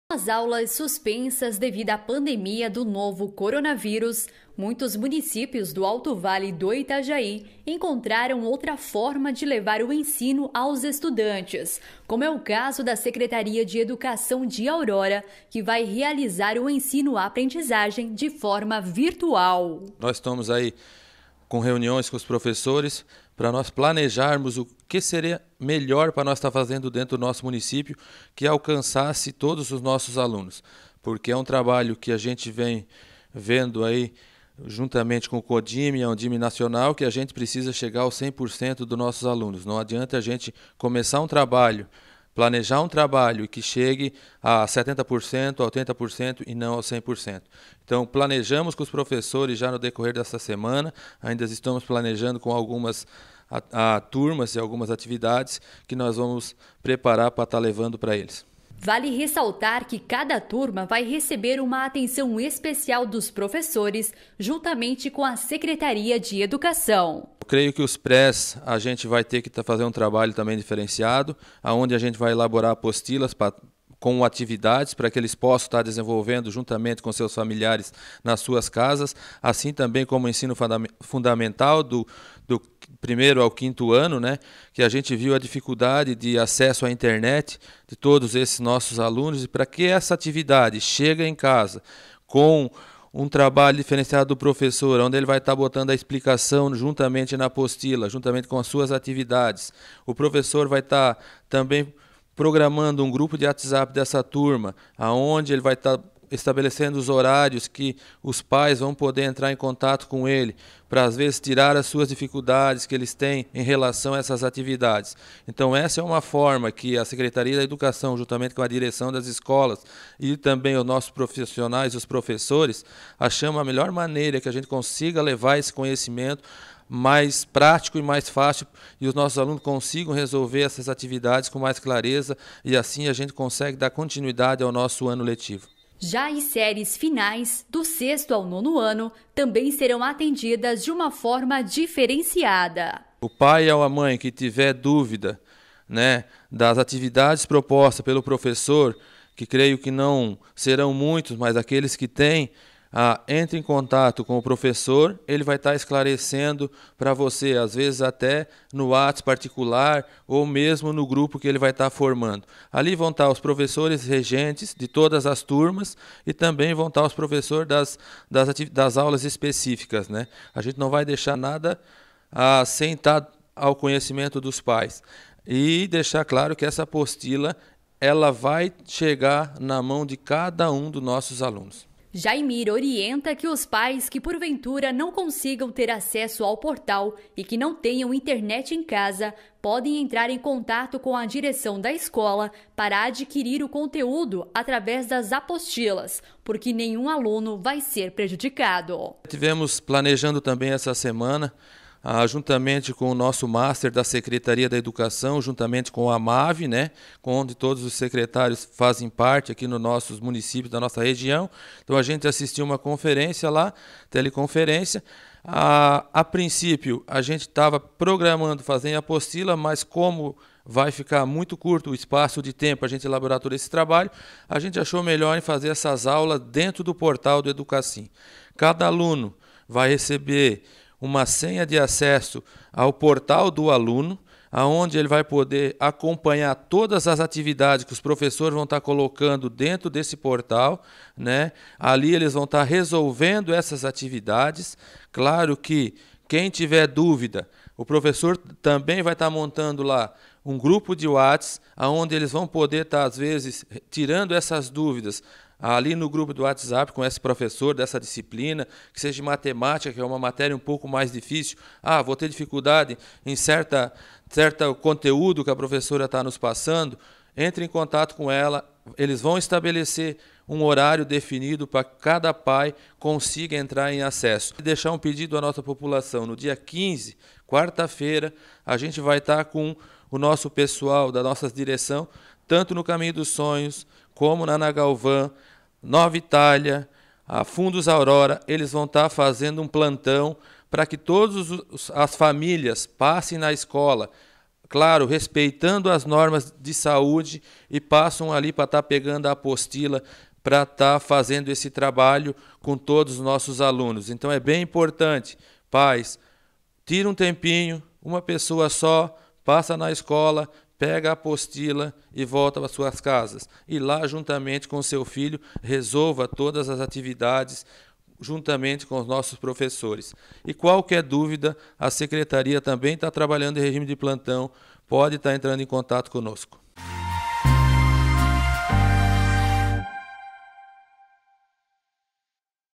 09/04/2020 - Secretaria da Educação de Aurora estará realizando ensino-aprendizagem de forma virtual
Faça parte da nossa programação!

Vale TV Online
A TV Online do Alto Vale